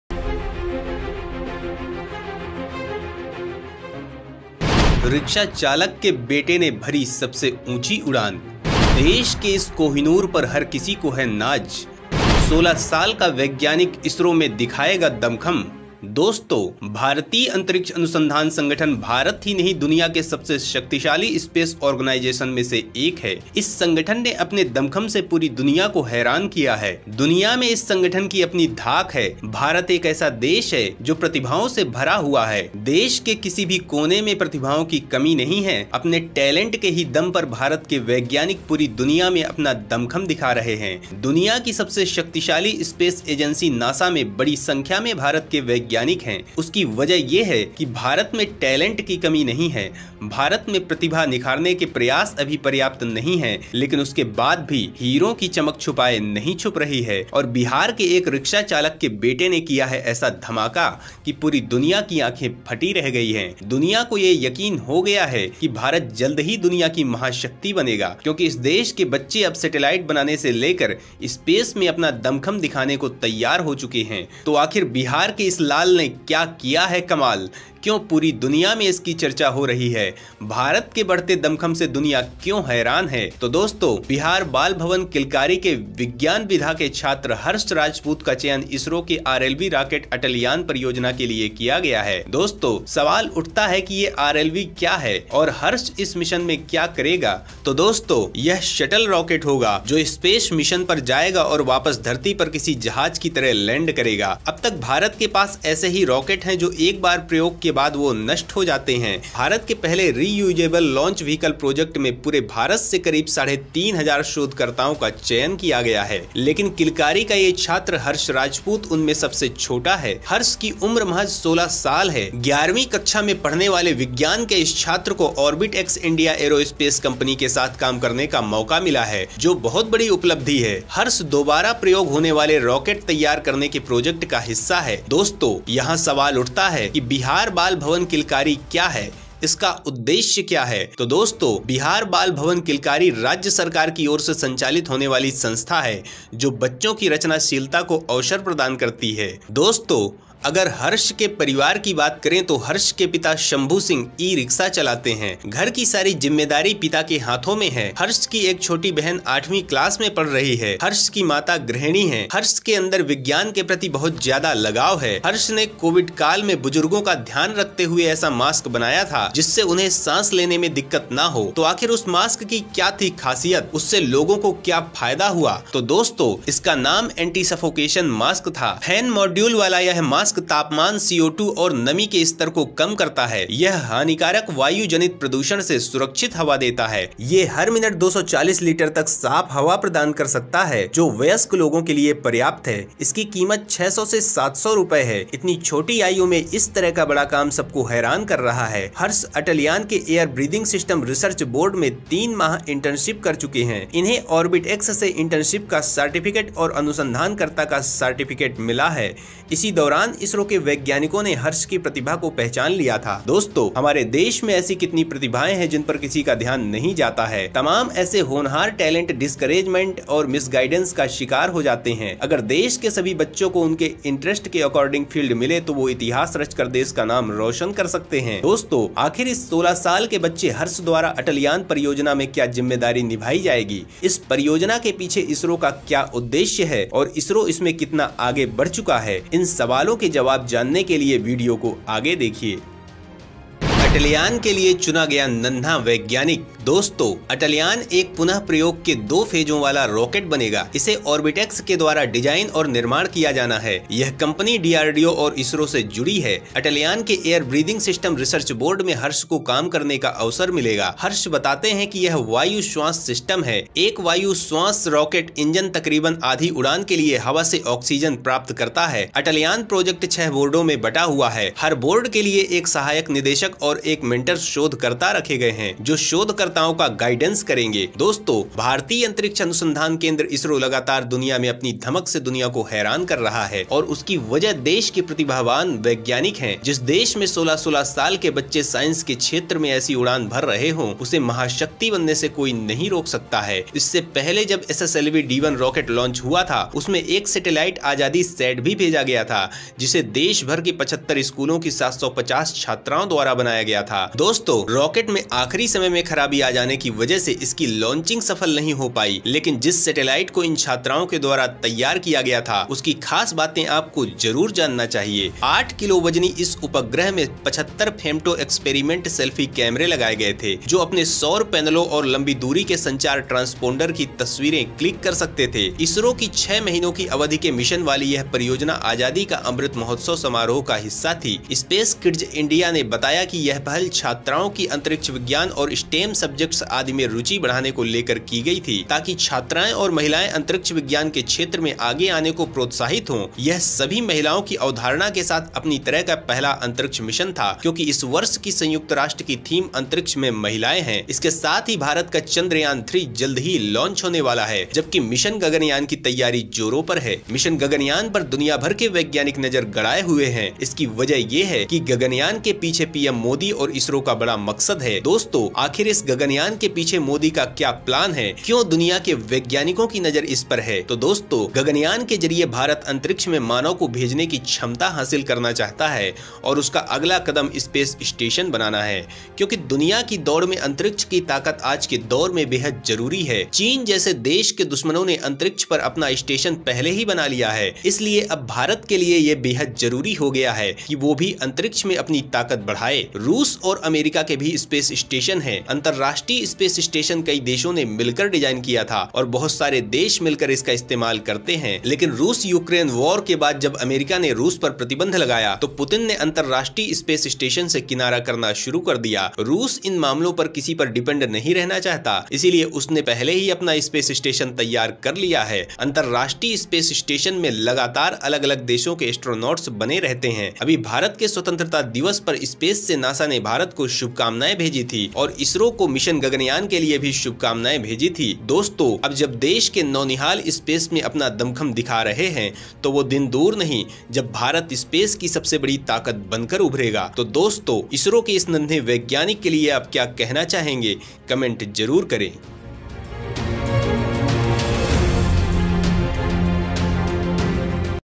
बिहार के रिक्शा चालक के बेटे ने इसरो में किया कमाल, riksha chalak ke bete ne ISRO me Kiya kamal
बिहार के रिक्शा चालक के बेटे ने इसरो में किया कमाल
bihar ke riksha chalak ke bete ne ISRO me Kiya kamal